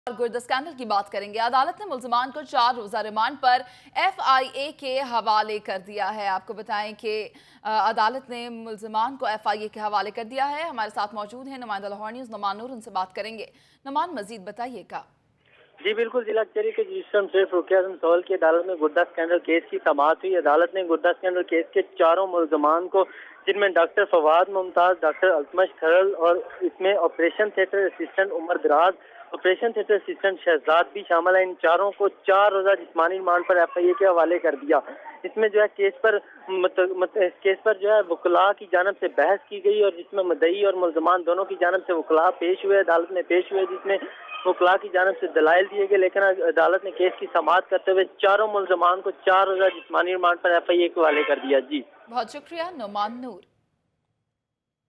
Illegal kidney transplant scandal: Court hands over suspects to FIA on 4-day remand
The hearing of case against doctors accused in illegal kidney transplant scandal has been proceeded today in Lahore High Court. The court has handed over suspects to Federal Investigation Agency (FIA).